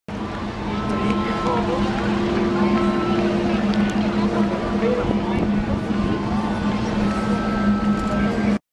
Lewis the Lion sees the Zocolo, Mexico City
Lewis the Lion overlooks the biggest square he's ever seen in his little life: the Zocolo, Mexico City's main square.
He's standing on the roof of the Cathedral for this fantastic view of the city.